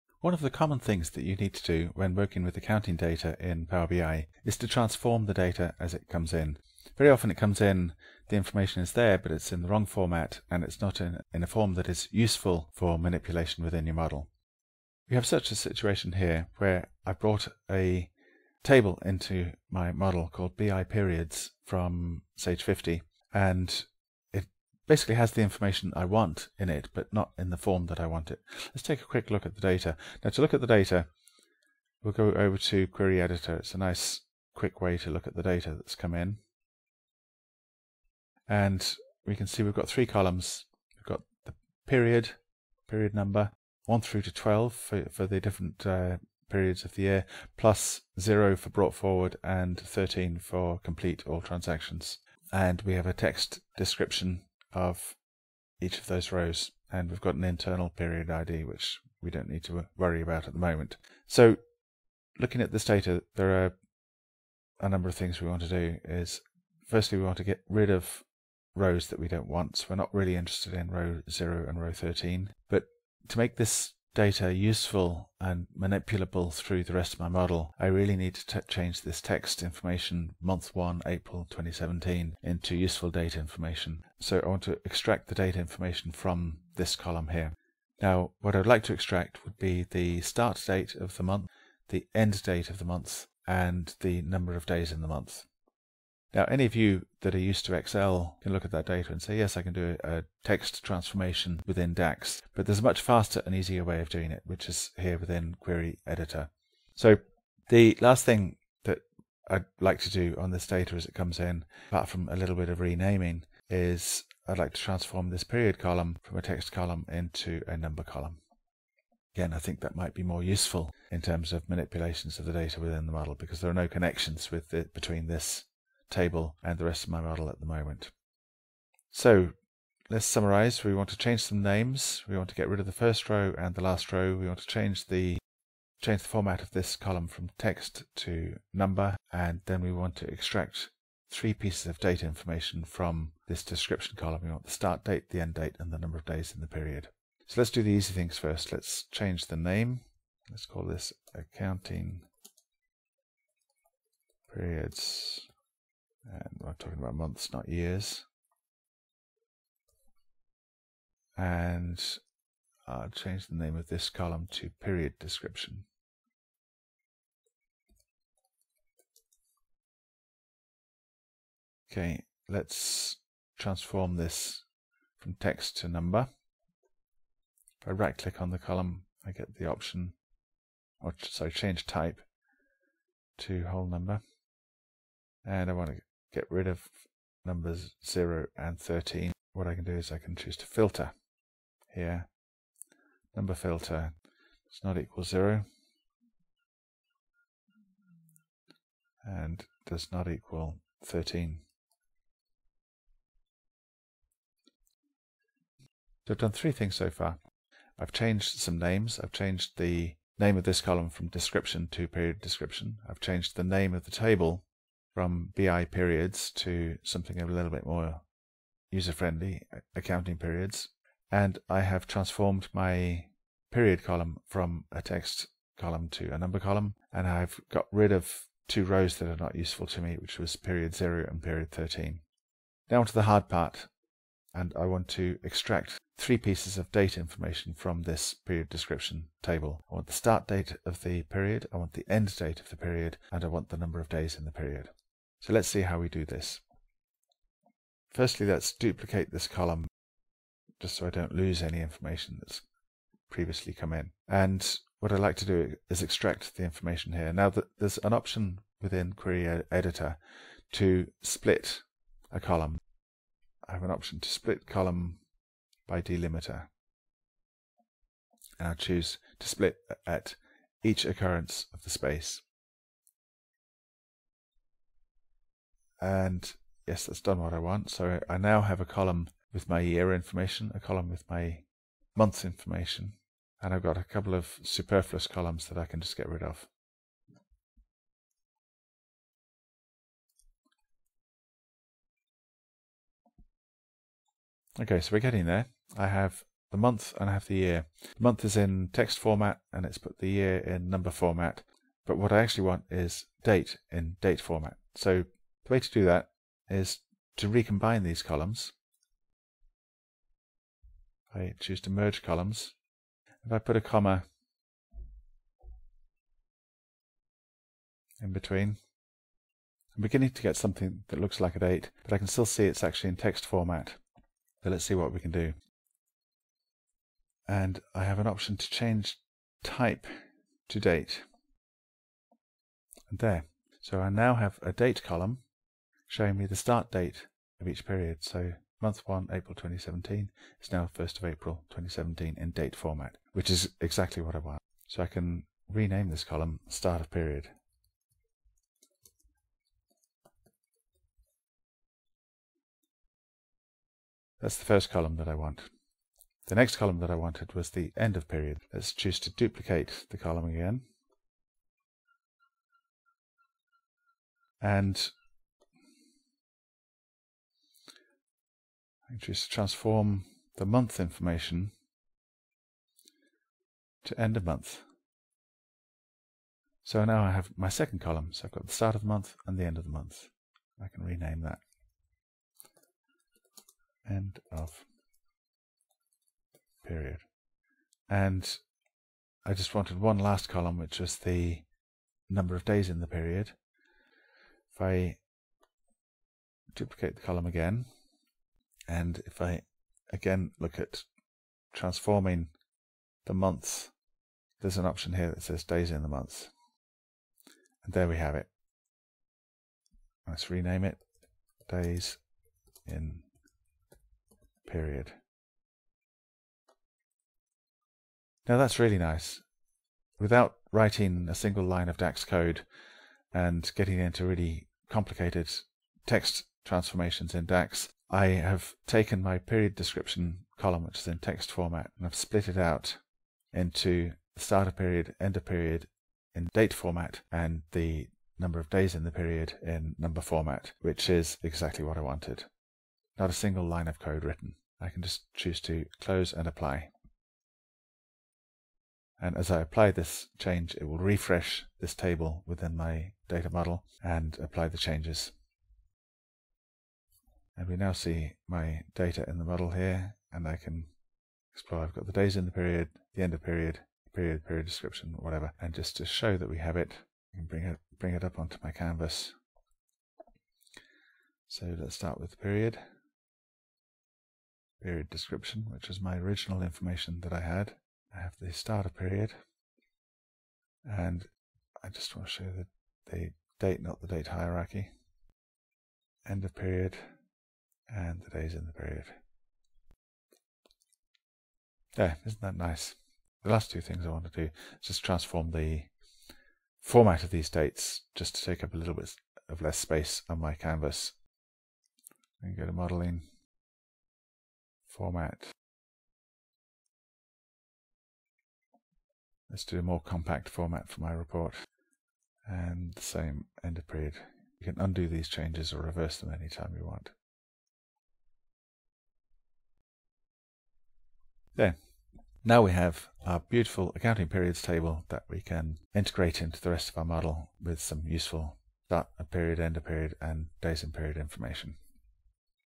Extracting dates from text using Power BI Query Editor
This video shows how to use the Power BI Query Editor to extract "Start of Period", "End of Period" and "Days in Period" from a Sage 50 text field that contains information in the form "Month1, April 2017".  Save hours of DAX coding with these few simple steps.